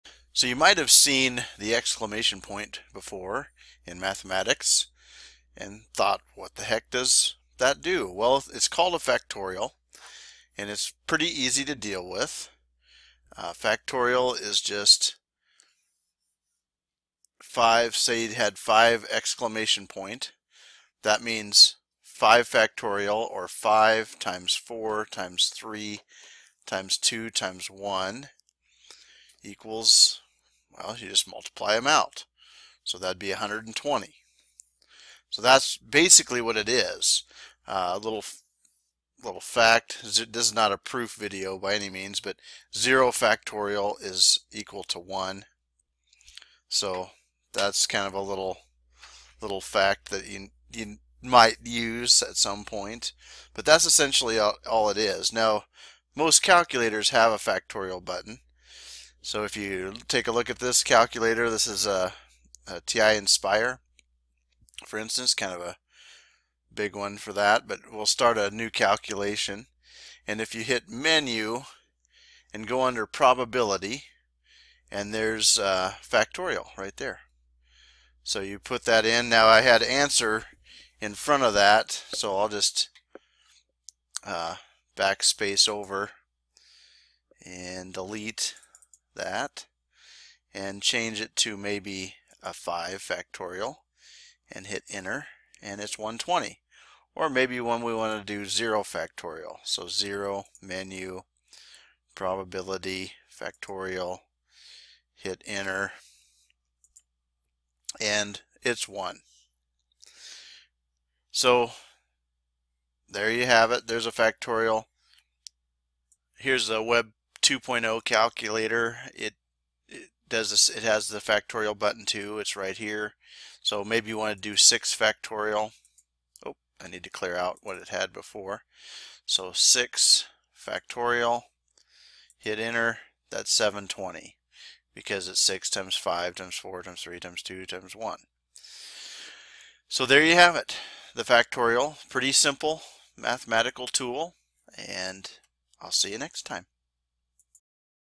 What does ! factorial mean in math
What does the ! mean anyway?  It means factorial.  So 5! is 5 factorial